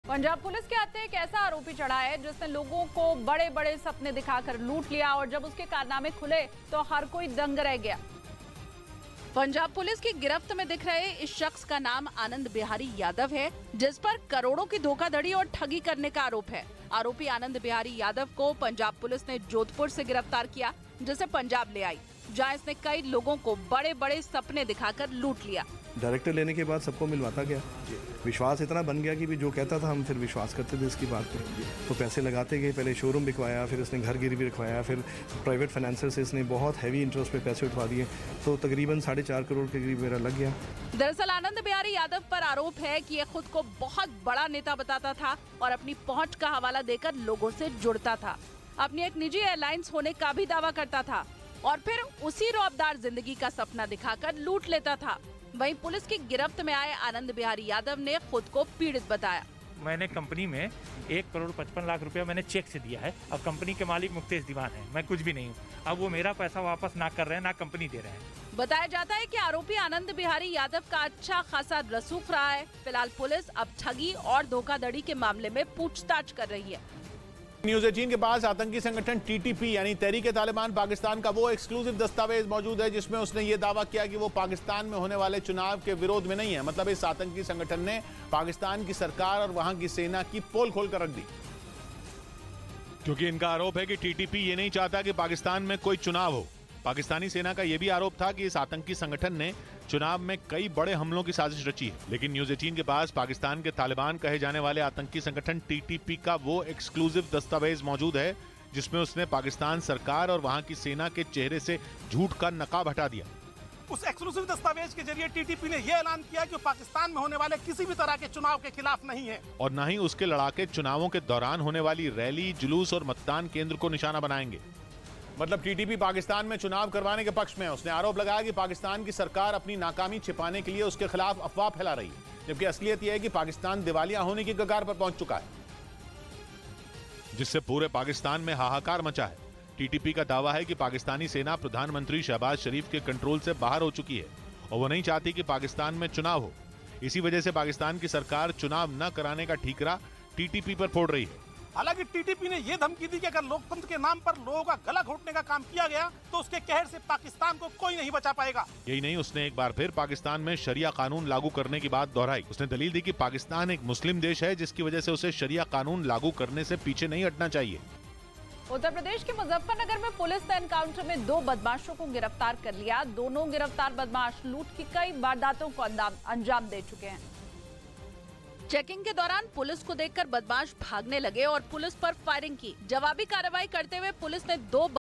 ऐसी ठगी आपने कभी नहीं देखी होगी? | Punjab Police | Rajasthan News | Jodhpur News | News18 Live
Punjab Police has caught such an accused…who looted people by showing big dreams and now Anand Bihari Yadav has been arrested by the police.
Punjab Police के हत्थे एक ऐसा आरोपी चढ़ा है जिसने लोगों को बड़े बड़े सपने दिखाकर लूट लिया और अब आनंद बिहारी यादव को पुलिस ने गिरफ्तार कर लिया है।

news18 live | news18 india live | live news | aaj ki taaja khabar | n18oc_states